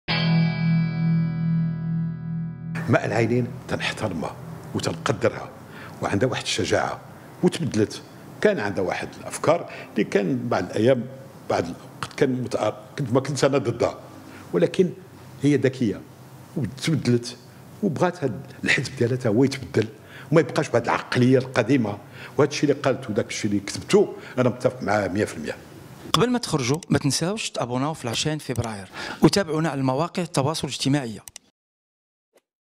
عيوش: ماء العينين امتلكت الشجاعة وغيرت كثيرا من مواقفها وتريد تغيير عقلية البيجيدي القديمة
فبراير تيفي | حصريـــــــــاً